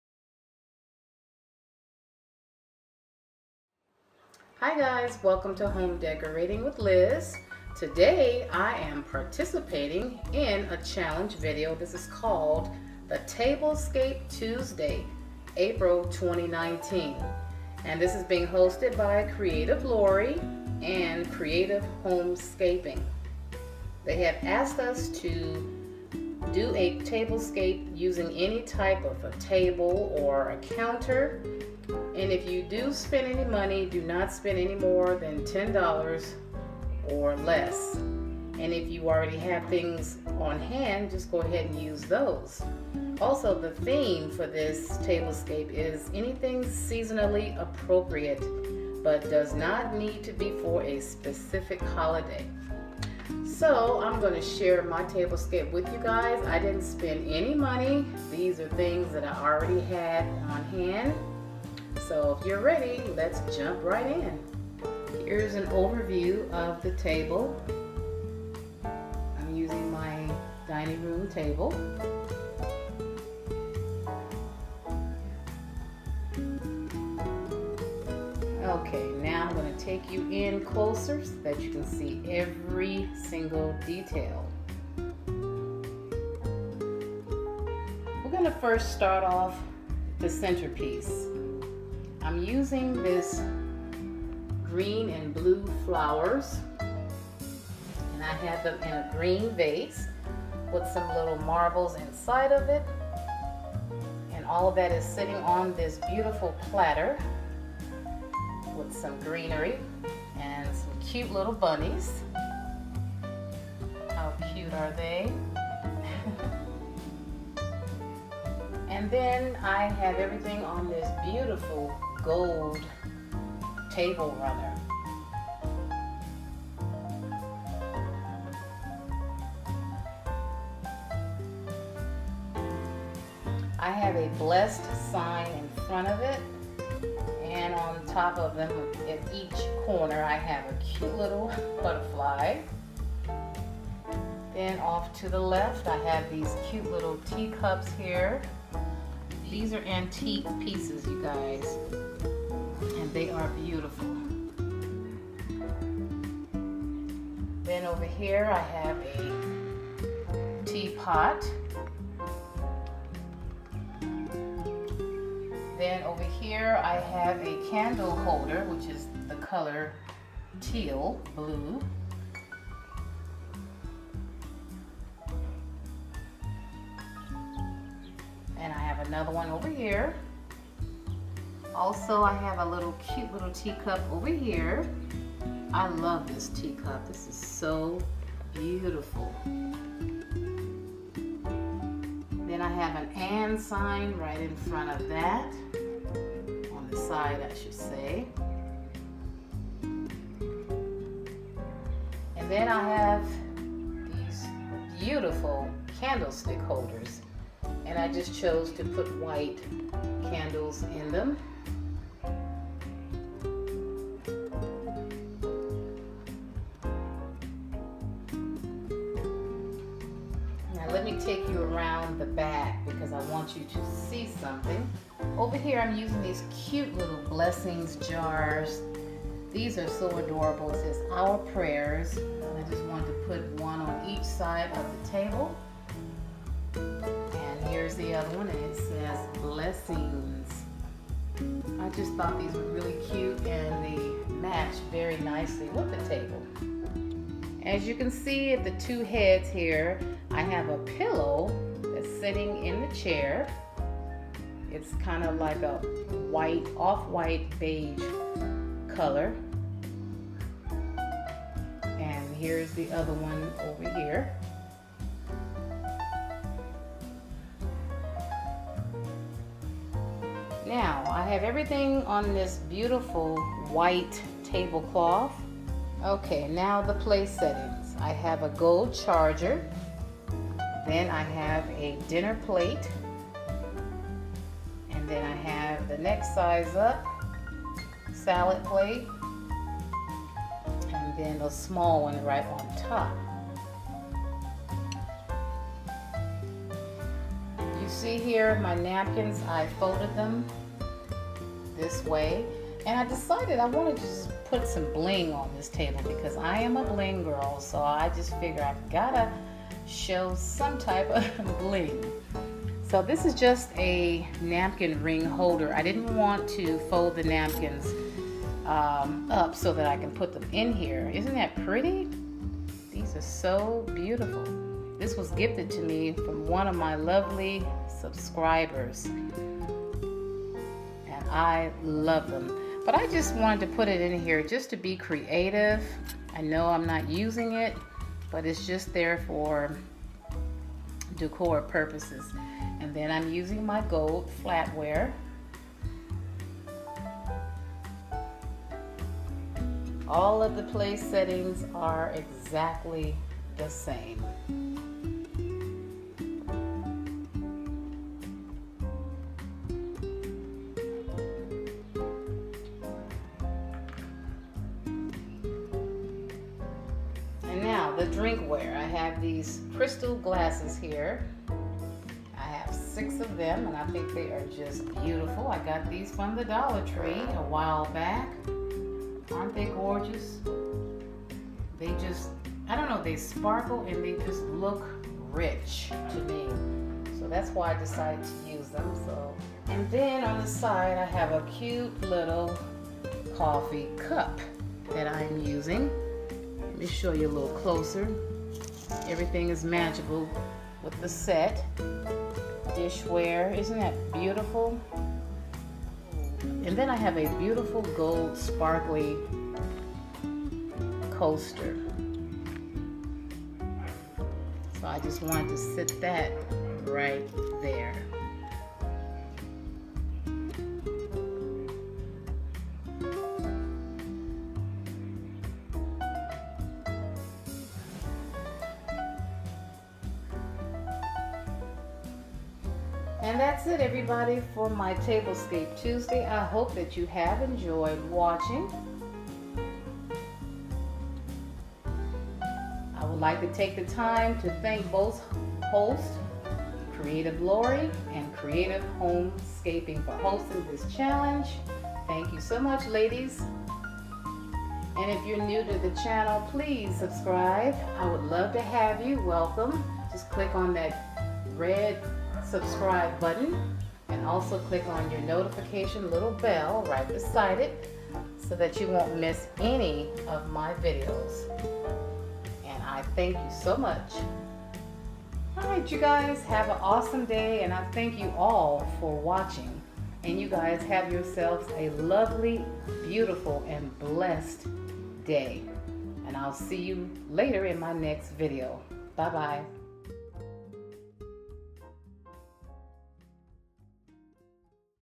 TABLESCAPE TUESDAY APRIL 2019 with CREATIVE HOMESCAPING & CREATIVE LAURI
Hello Friends! Today's video I'm sharing and participating in a challenge. In this challenge I will show my seasonal tables cape. Hope you enjoy!

Send Me Friend Mail:
HomeDecoratingwithLiz
39341 Thames Court

"A Special Thank You" to Pam & Lauri for hosting this challenge!